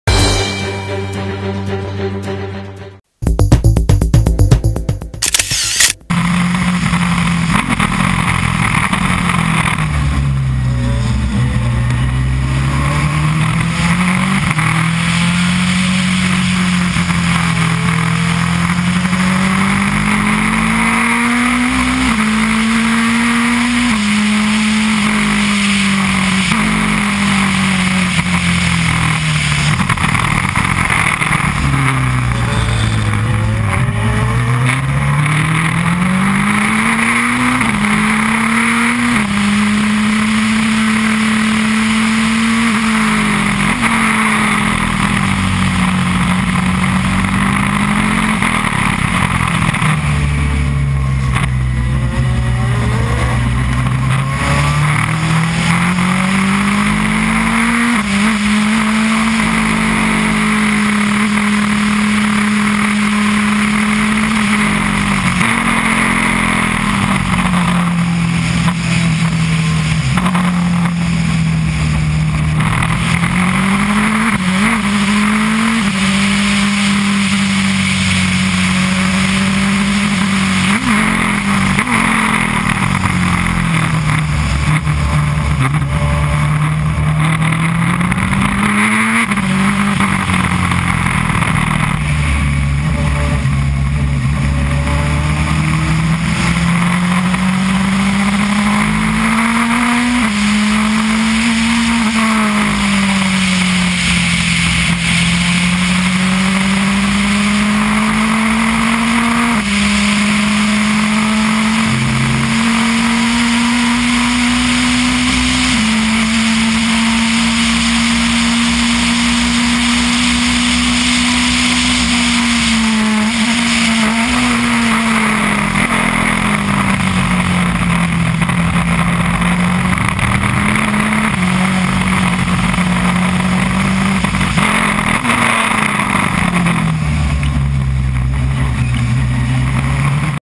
CAMERA CAR FERRARI GT2 , CIRCUITO PORTIMAO (PORTOGALLO)
by Marco Petruzzelli
Camera car Ferrari GT2 del Pilota Lorenzo Casè del Team Black Jack durante i test nel ciurcuito di Portimao in Portogallo.